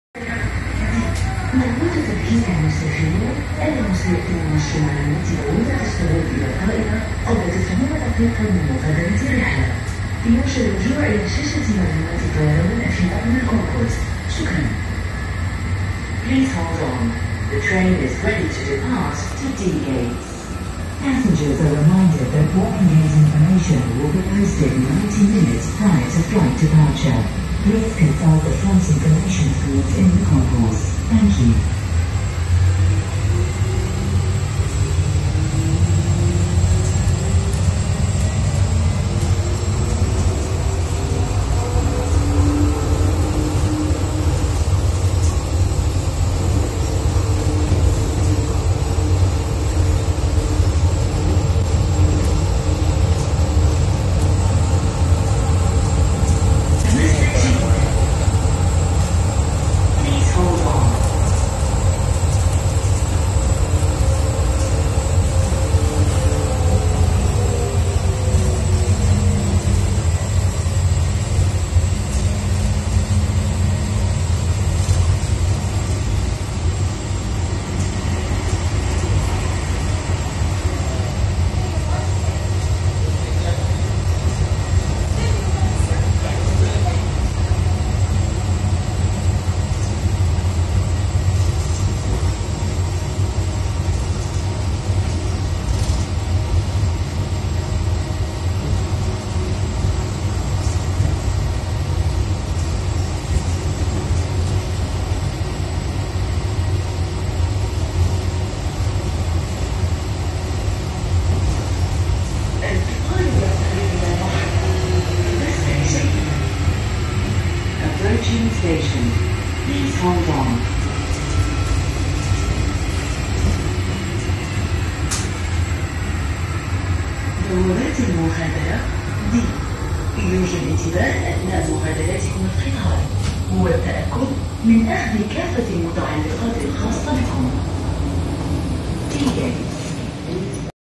Dubai inter terminal transit unclear video...trying YouTube
Very bad video from mobile phone in Dubai airport